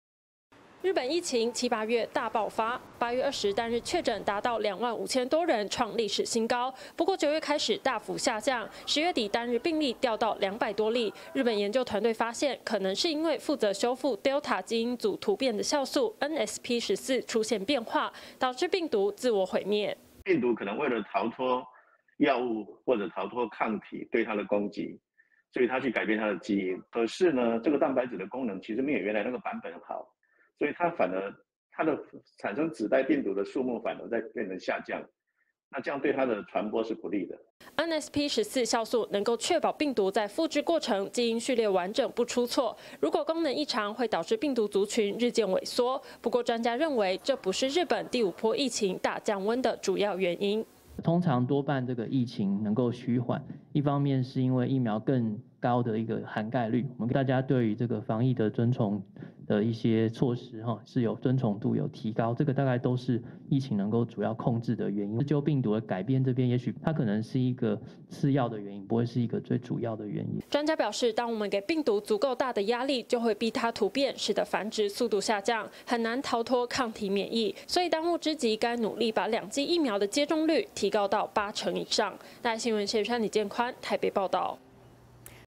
病毒滅絕？日本第5波疫情快速降溫
日本COVID-19第5波疫情呈現快速降溫傾向，疫苗接種率提高，以及民眾願意配合防疫奏效等被視為可能原因。國立遺傳研究所和新潟大學的研究團隊30日公布彙整研究指出，其中一個原因，也可能是負責修復Delta變異株基因組突變的酵素「nsp14」發生變化，來不及修復突變，導致病毒絕滅。

日本疫情七八月猛爆式成長，820單日確診達到2萬五千多人創歷史新高，不過9月開始大幅下降，10月底單日病例掉到百多例，日本研究團隊發現，可能是因為負責修復Delta基因組突變的酵素nsp14出現變化，導致病毒自我毀滅。

台大兒童醫院院長 黃立民：「病毒可能為了逃脫藥物，或者逃脫抗體對它的攻擊，所以它去改變它的基因，可是呢 這個蛋白質的功能，其實沒有原來那個版本好，所以它反而它產生子代病毒的速度，反而在變得下降，那這樣對它的傳播是不利的。」

Nsp14酵素能夠確保病毒在複製過程，基因序列完整不出錯，如果功能異常，會導致病毒族群日漸萎縮，不過專家認為，這不是日本第五波疫情大降溫的主要原因。

醫療應變組副組長 羅一鈞(11/1)：「通常多半這個疫情能夠趨緩，一方面是因為疫苗更高的一個涵蓋率，大家對於防疫的遵從的一些措施，疫情能夠主要控制的原因，就病毒的改變這邊，也許它可能是一個次要的原因，不會是一個最主要的原因。」

專家表示，當我們給病足夠大的壓力，就能逼它突變使得繁殖速度下降，很難逃脫抗體免疫，所以當務之急，該努力把兩劑疫苗接種率提高到八成以上。

謝昀珊 李建寬 台北報導